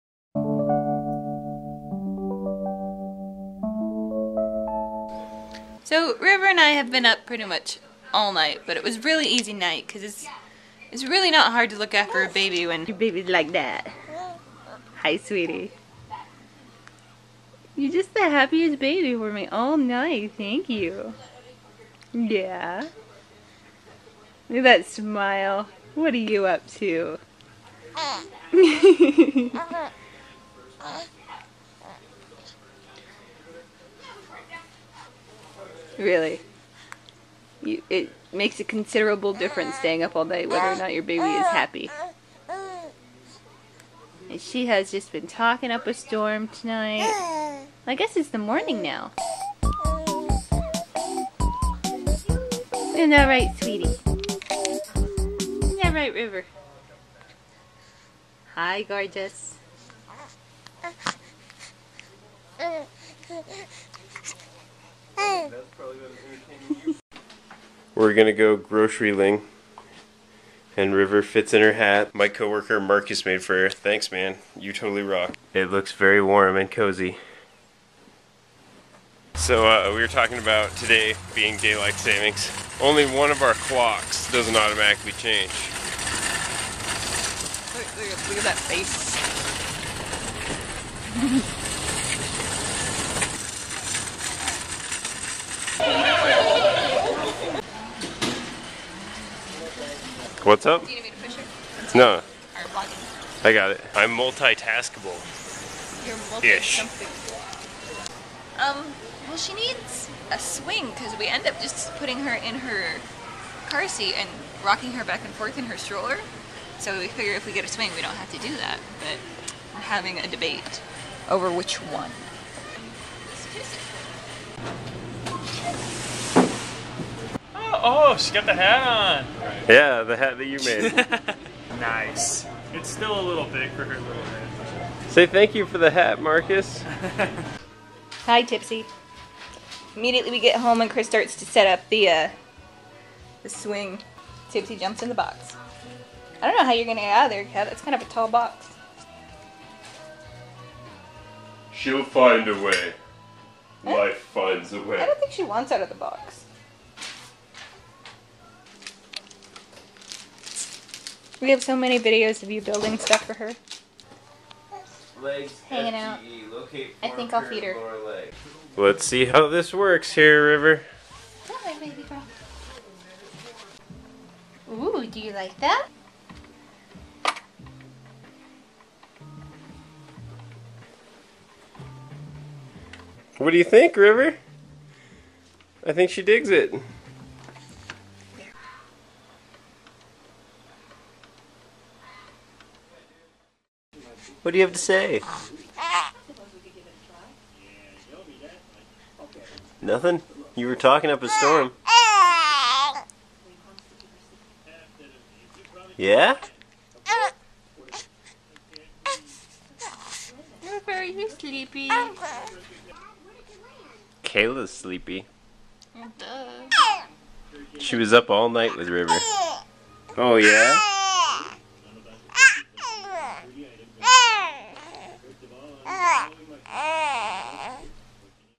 SWING
11-05-10
River kept Kayla up late. But at least she was full of smiles and just wanted to be talked to, and played with.

11-06-10
After I got off work, Kayla and I hung out for a while, than gathered River for some shopping. We hit a few stores looking for a baby swing, and than hit the Rosaures for some food stuffs. We showed off River to Marcus, sporting the hat he croshayed for her. Thanks Man!

11-07-10
Barely filmed today, but I had to leave this little clip on, she's just too cute.